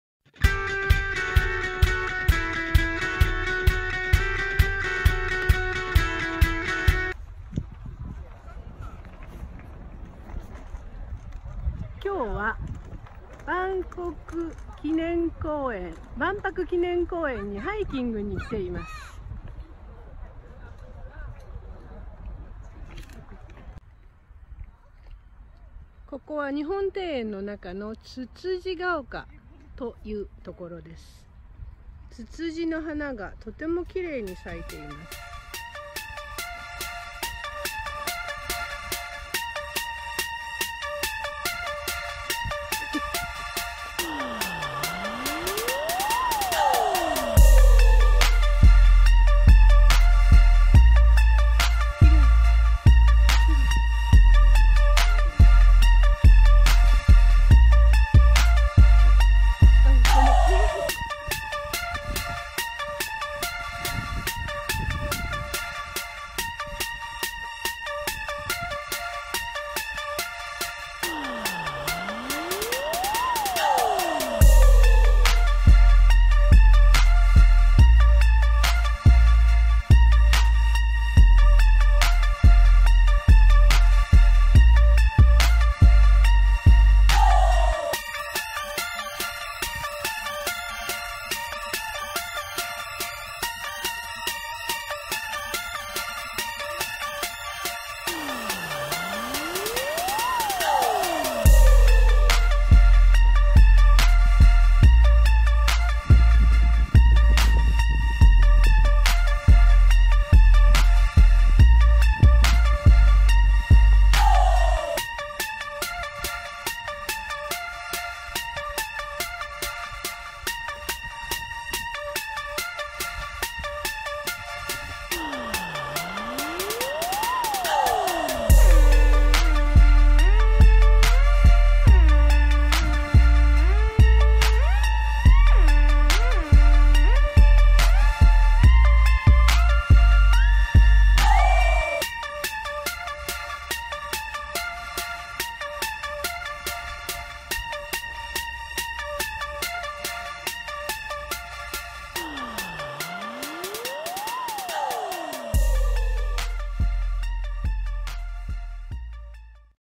美しい日本の風景 万博公園 つつじ
万博記念公園の中にある日本庭園
つつじの花がきれいでした。